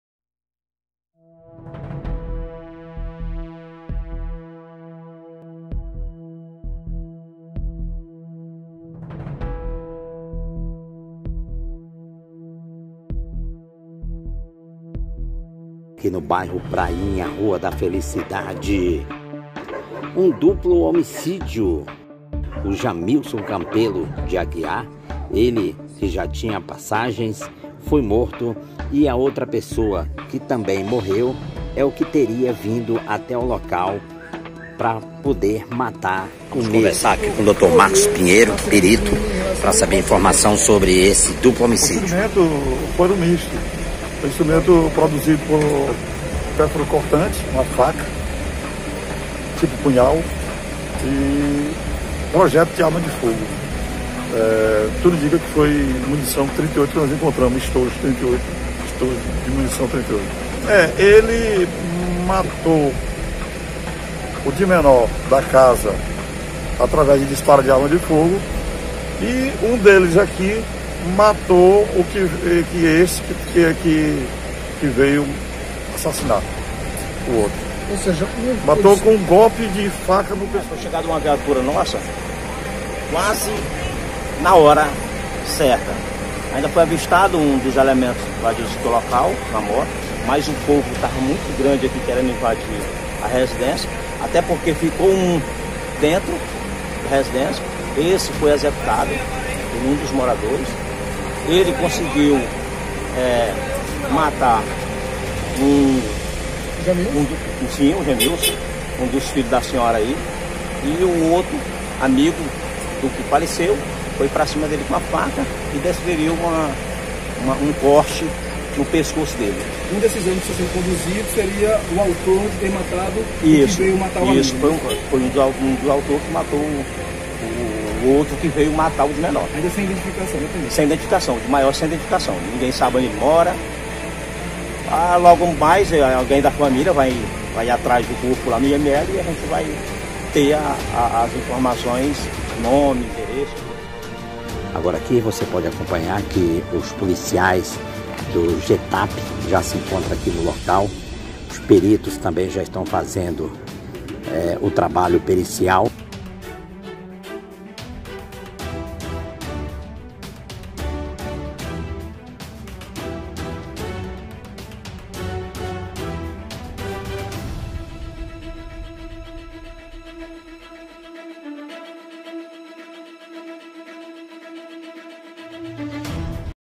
Duplo homicídio na região da Prainha zona sul da capital - 23/07/2022 - TV CAPITAL TERESINA
Reportagem: JOSELITO ANDRADE O repórter do povão